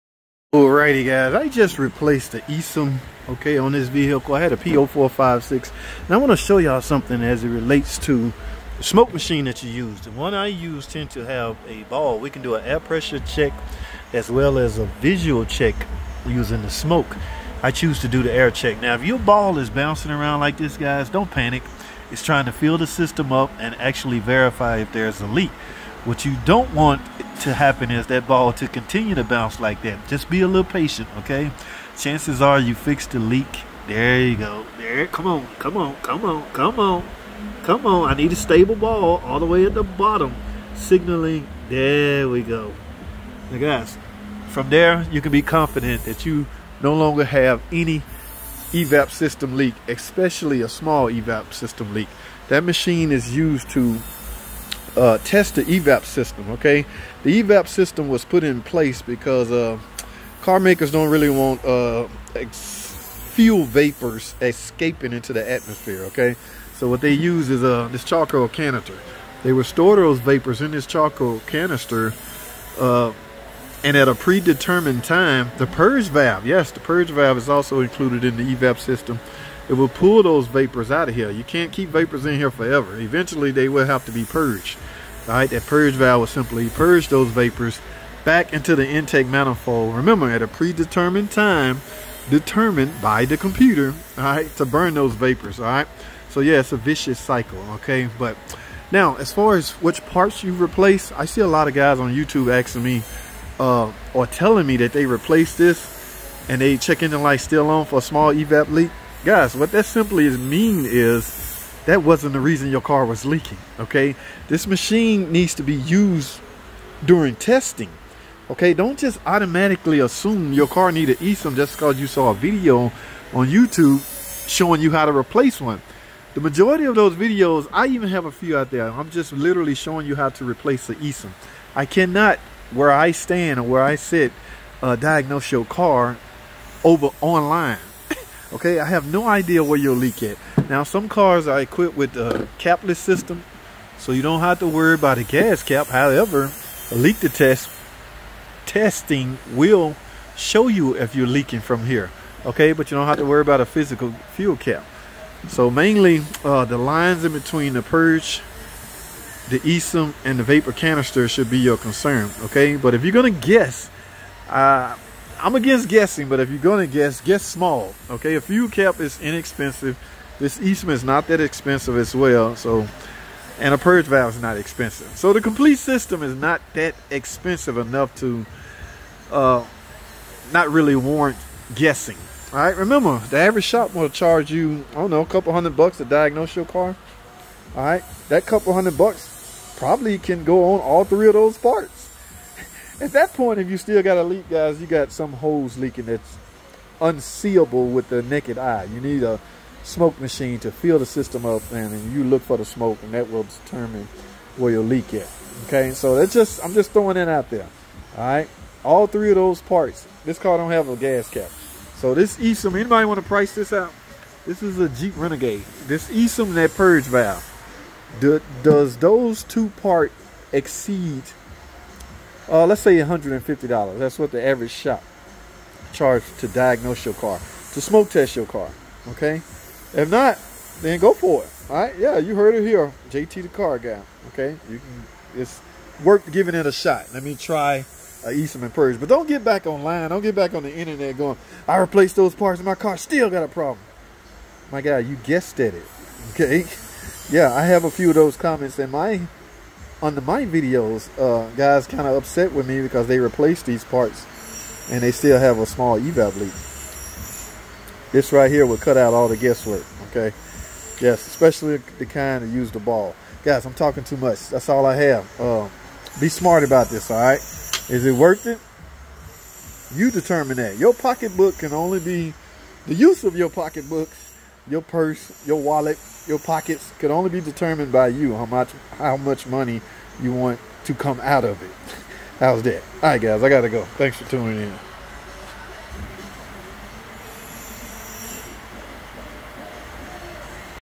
The Evap System Explained. Fault Code P0456 and this ESIM.
Jeep Renegade Check Engine Light On with P0456